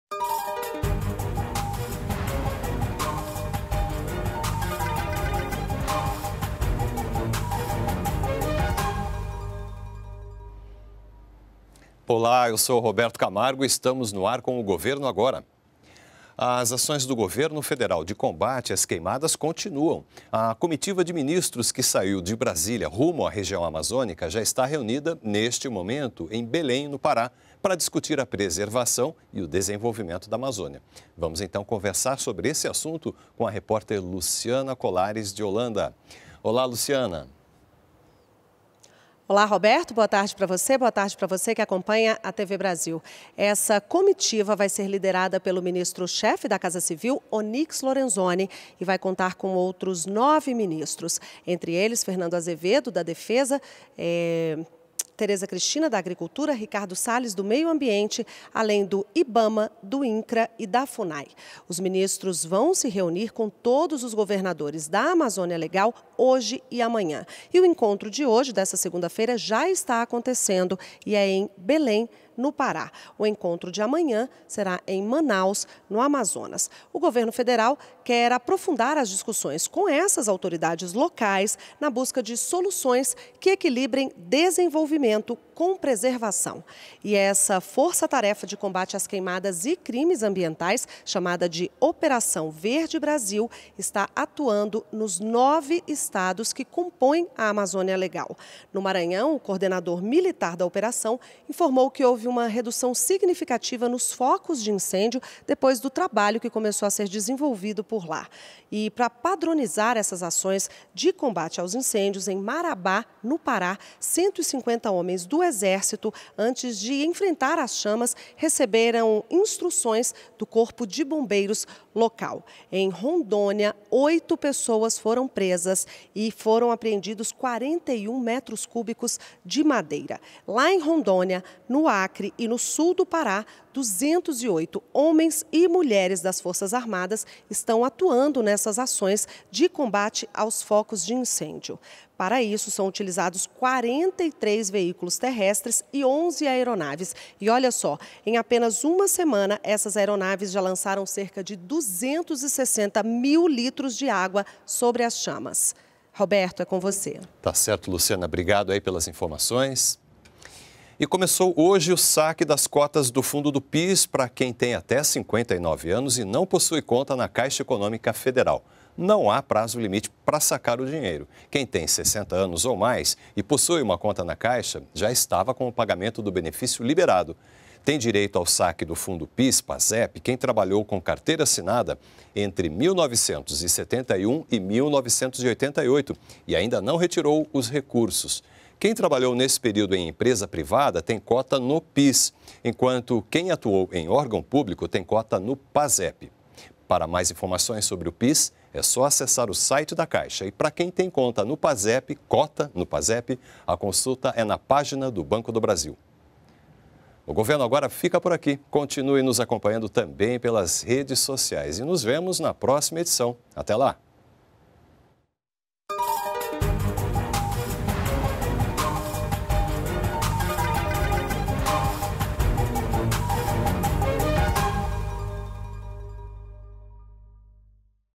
Ministros se reúnem para discutir a preservação e o desenvolvimento da Amazônia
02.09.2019 - Comitiva de ministros que saiu de Brasília para a região Amazônica está reunida em Belém - PA, para discutir a preservação e  o desenvolvimento da Amazônia, e ainda, começa hoje os saques das cotas do fundo do PIS, para quem tem até 59 anos e não possui conta na Caixa Econômica Federal.